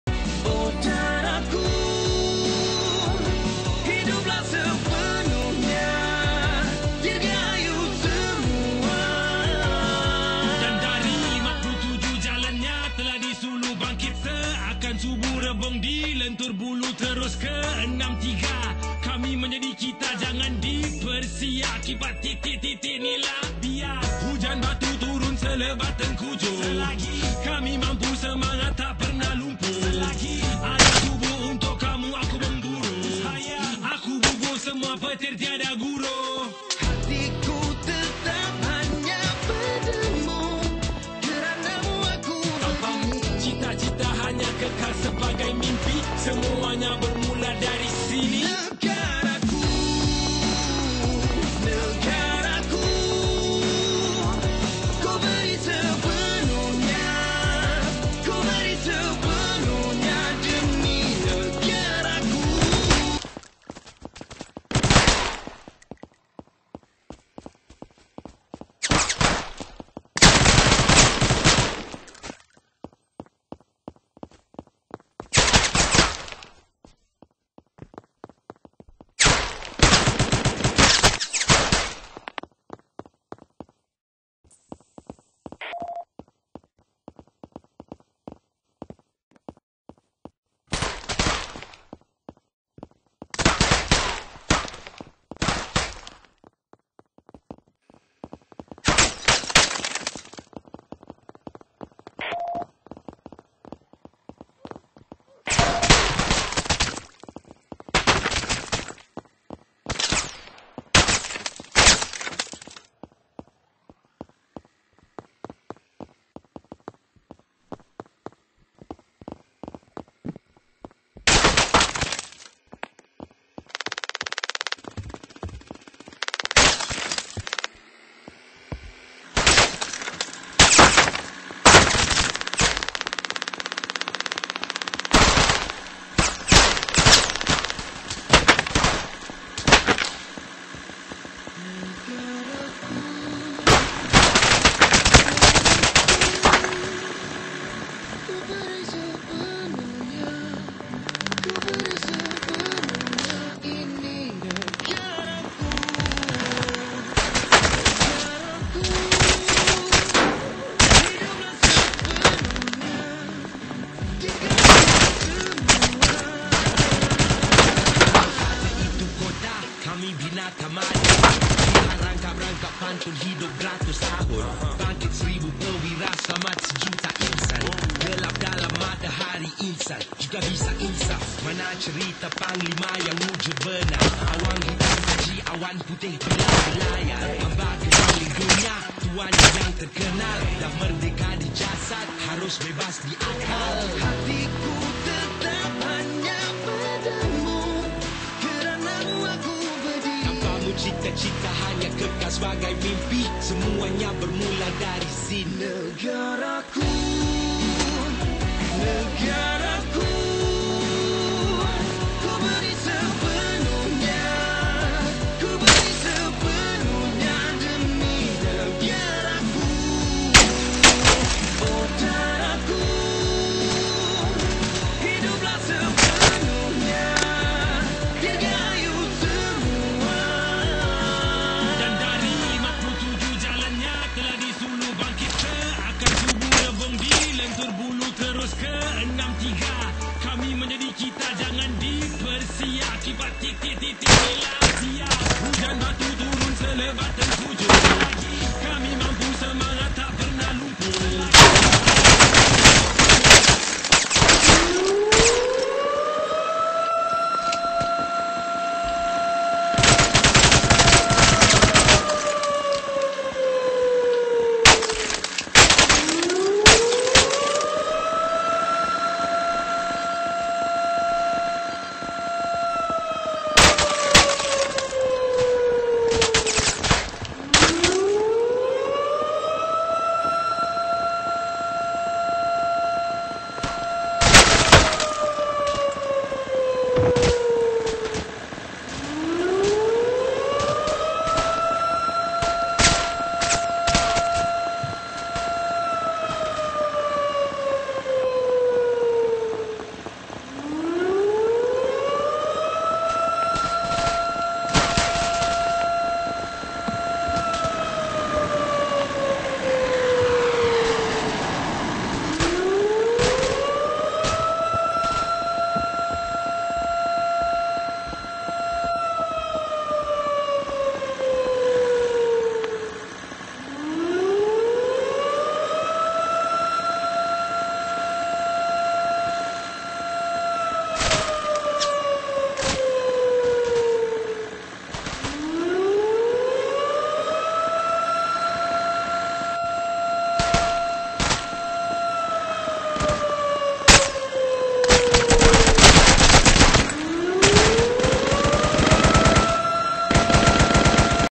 Special Operation Trailer [Hutan V.2]
Trailer Special Unit PDRM
Host:
1. SayaPolis
2. Kookie_Adriana
Actor:
1. Dreadlius123
2. kingzLexx
3. syamsulyusof_Imran
4. DuckyyyyYT
5. jaking149
6. roplex170
7. KuykieTam
8. taufikhidayatpro
9. fal7452